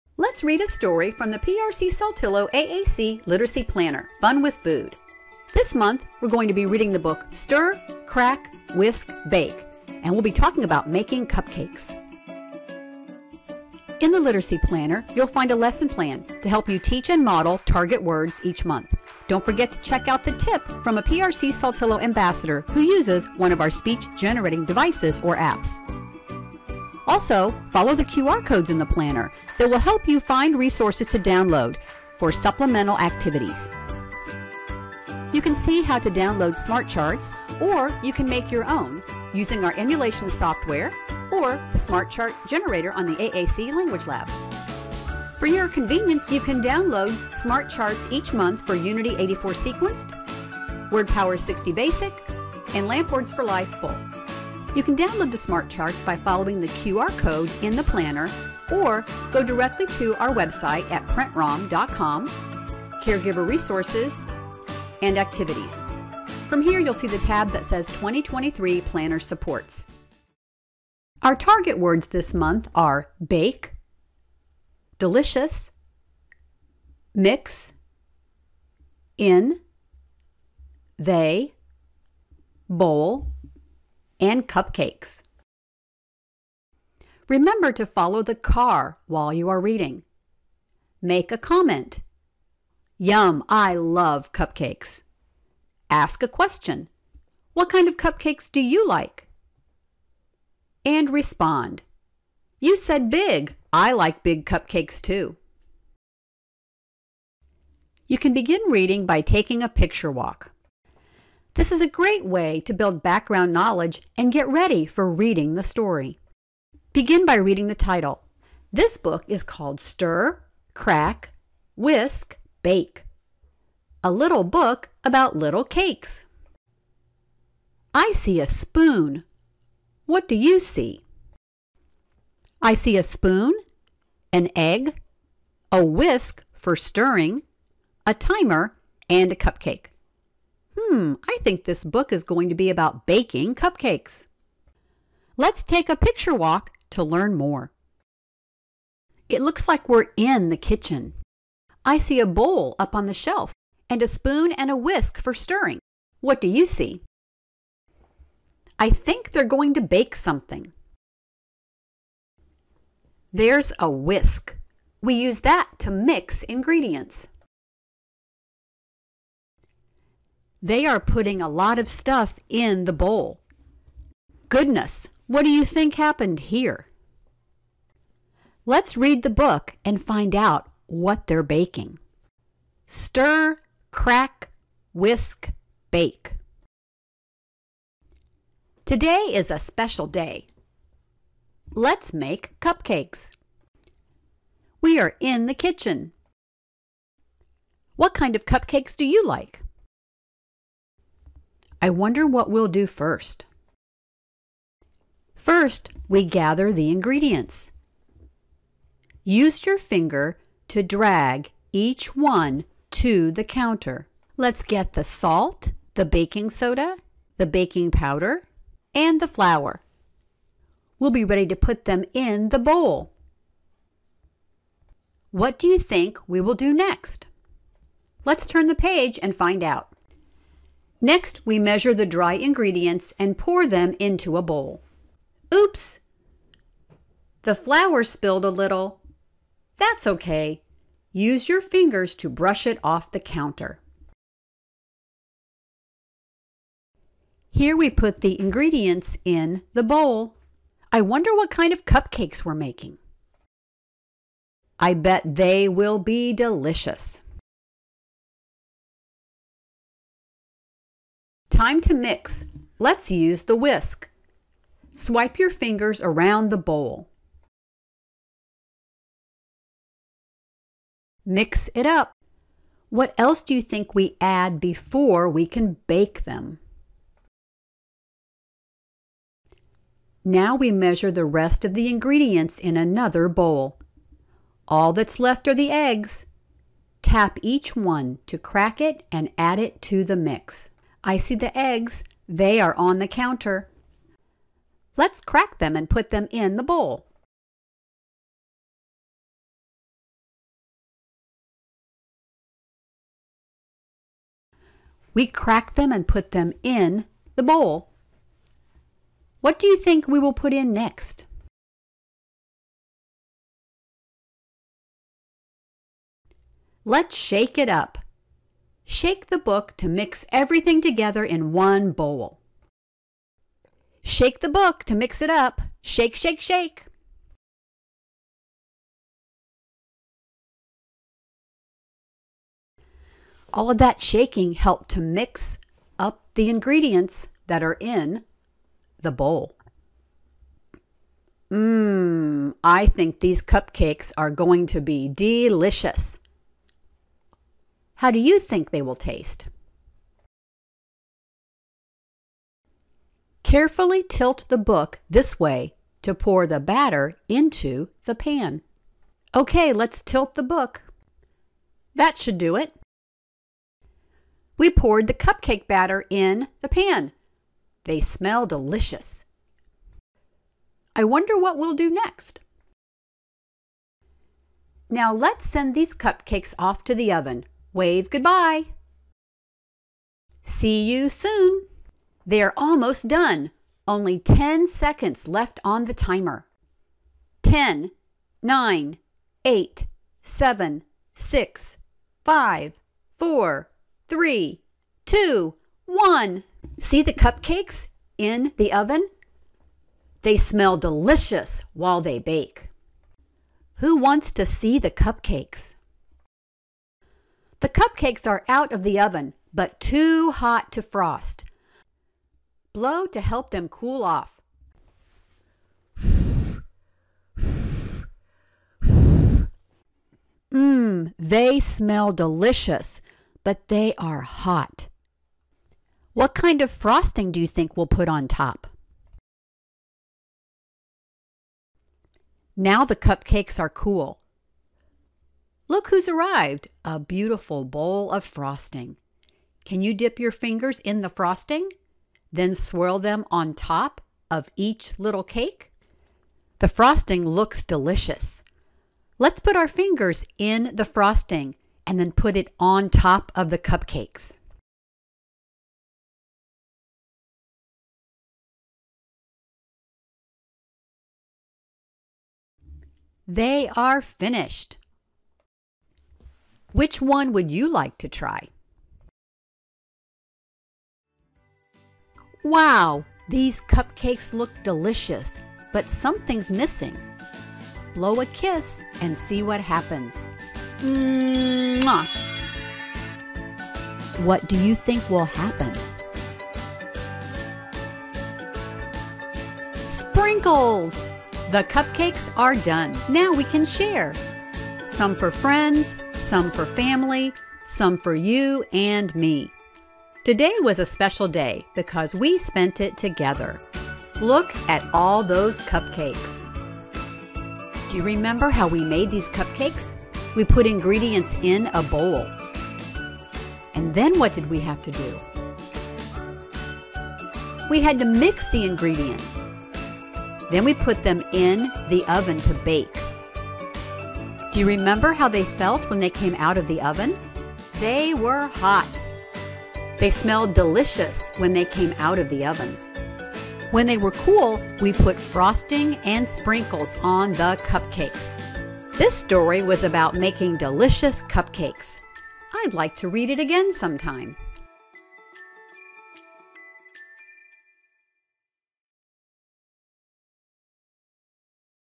January 2023 Stir Crack Whisk Bake
Are you ready to read with us in 2023? The 2023 PRC-Saltillo AAC Literacy Planner is here. Enjoy this month's story, Stir, Crack, Whisk, Bake by America's Test Kitchen and Maddie Frost. This read-aloud video provides an opportunity for a shared reading activity using 5-6 target words with a smart chart to help you model these words. You’ll take a picture walk to build background knowledge and then listen to the story.

0.00  Introduction to the Literacy Planner
1:10 Story Intro and Picture Walk
3:02  Read the Story

Please pause the video as you listen to the story to interact with your AAC communicator. You can ask your PRC-Saltillo AAC Consultant for a free print copy or download a copy from our

Note: This video example uses LAMP Words for Life Full vocabulary symbols but you can read the story no matter what language system you are using. Download smart charts for LAMP WFL, Unity 84 sequenced, or WordPower 60 Basic or make your own using our free PASS software (download or the smart chart generator on the AAC Language Lab.